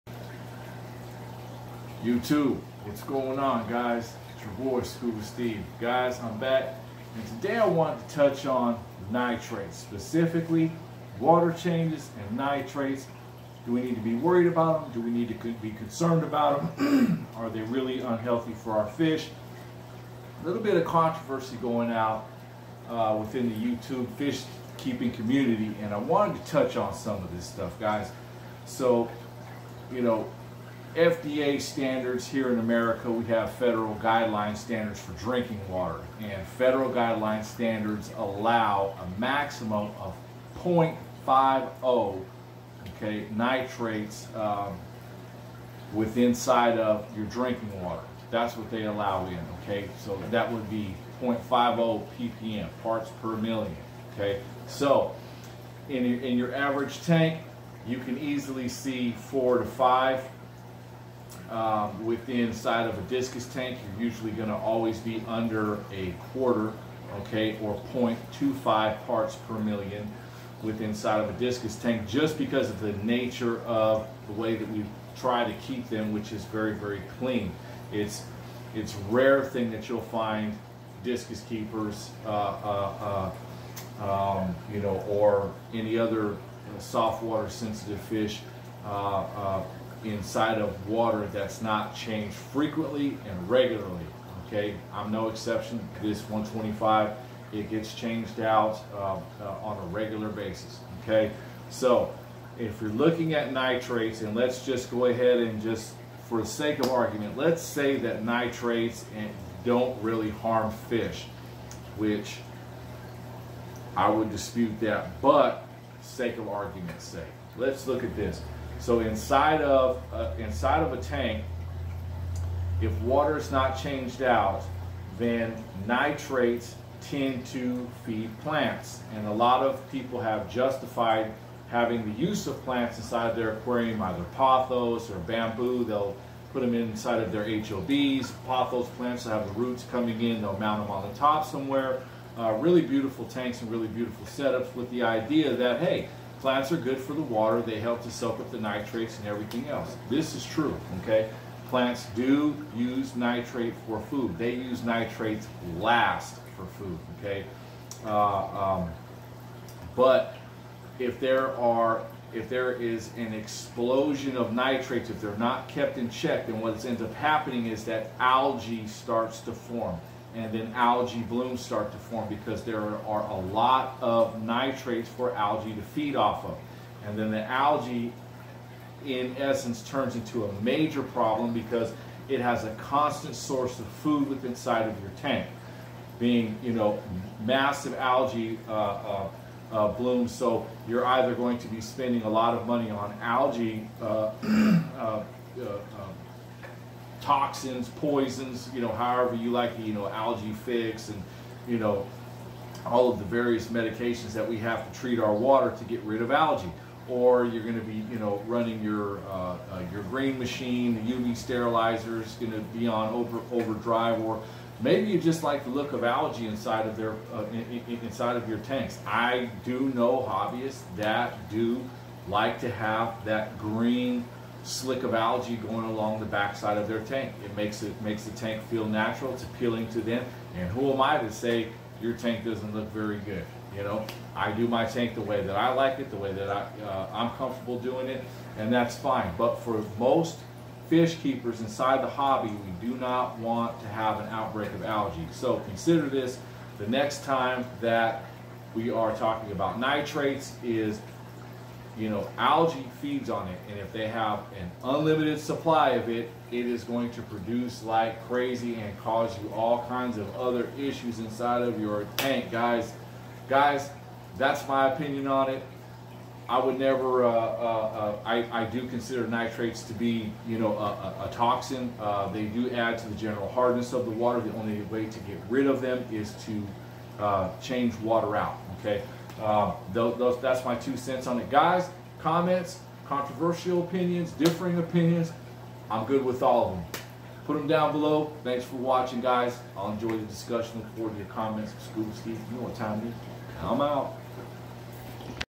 Nitrates harmful to your aquarium?
Another look at the effects of nitrates inside of our tanks!  The debate goes on